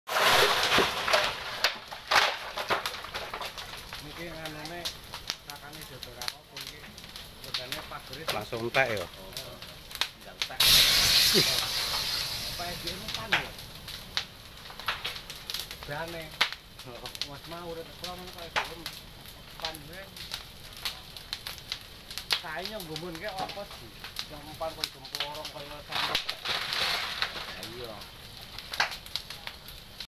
Ditinggal Ke Tempat Hajatan, Rumah Warga Wadas Purworejo Dilalap Sijago Merah 30 November 2020
Kebakaran melalap rumah Senen, warga Desa Wadas, Kecamatan Bener, Kabupaten Purworejo, Jawa Tengah, Senin (30/11/2020) pukul 20.30 WIB.
Saat kejadian pemilik rumah sedang membantu prosesi hajatan salah satu saudaranya.
          Kepala Desa Wadas, Fahri Setyanto membenarkan adanya musibah kebakaran tersebut.